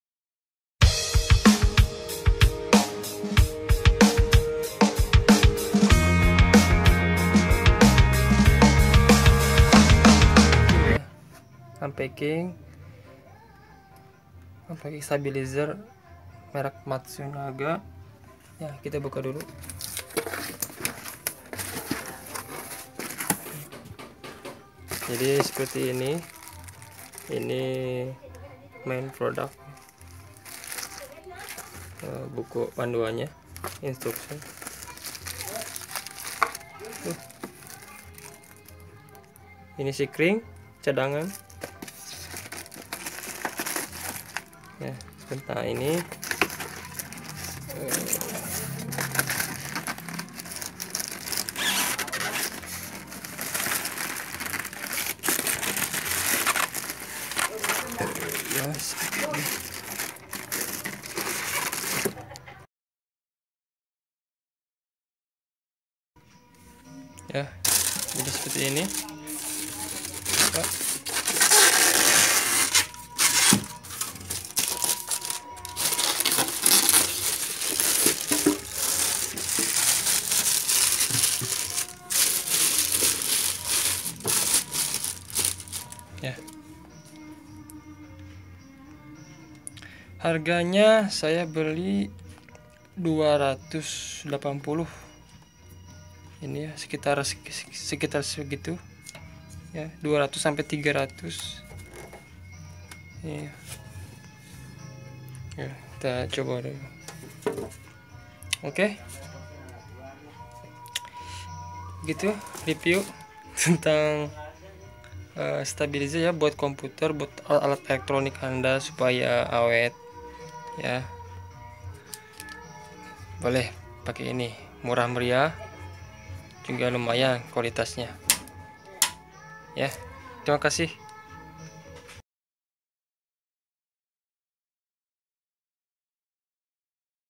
UNBOXING/UNPACKING STABILIZER MATSUNAGA - #1
Terima kasih sebelumnya & untuk request/mendukung channel ini silahkan klik link dibawah ini guys. :-)

Kali ini saya ingin unpacking stabilizer merek matsunaga.
mohon maaf jika video yang saya upload berkualitas jelek dan cara penyampaian saya kurang dipahami.
saya hanya ingin sharing kepada teman2 yang ingin membeli stablizier murah dan berkualitas standard.
terima kasih atas perhatiannya selamat menyaksikan..
kritik dan saran dipersilahkan comment di bawah.

Sampai jumpa di video unboxing-unboxing selanjutnya.
Salam ykyriky